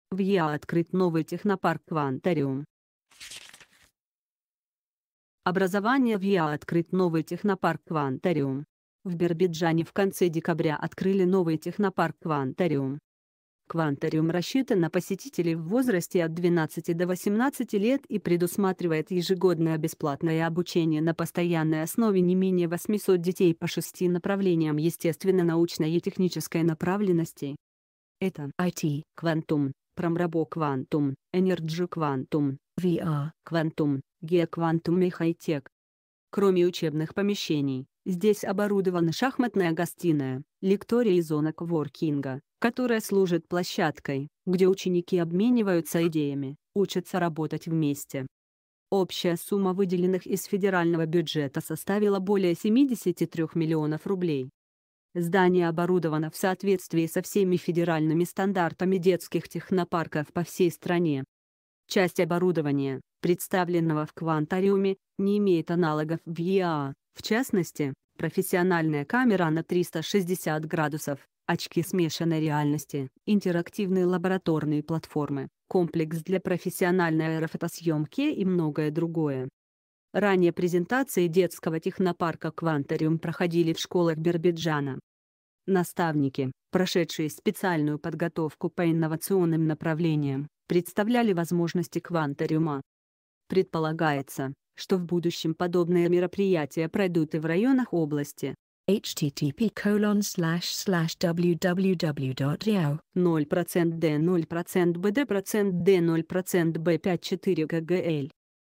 В ЕАО открыт новый технопарк Кванториум
В ЕАО открыт новый технопарк Кванториум Образование В ЕАО открыт новый технопарк Кванториум В Биробиджане в конце декабря открыли новый технопарк Кванториум . Кванториум рассчитан на посетителей в возрасте от 12 до 18 лет и предусматривает ежегодное бесплатное обучение на постоянной основе не менее 800 детей по шести направлениям естественно-научной и технической направленностей . Это IT-квантум, Промробоквантум, Энерджиквантум, VR-квантум, Геоквантум и Хайтек . Кроме учебных помещений, з